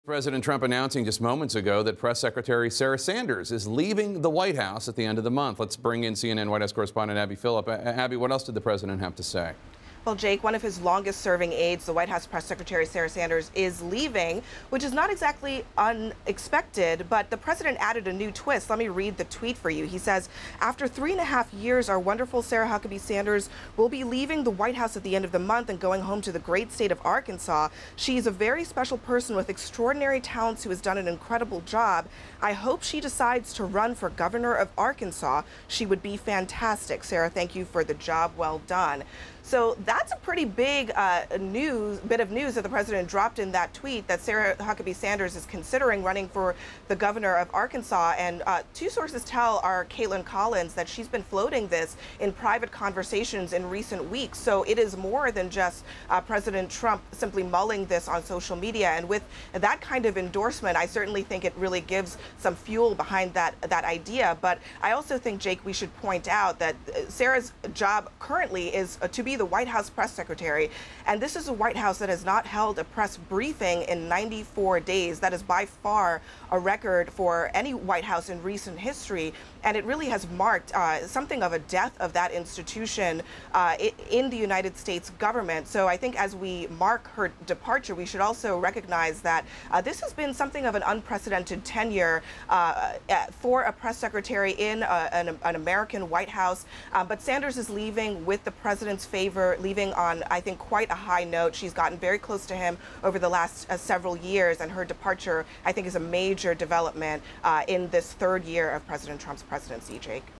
Trump tweets that Sarah Sanders is leaving White House
President Donald Trump tweeted that press secretary Sarah Sanders will be leaving the White House after more than three years of service.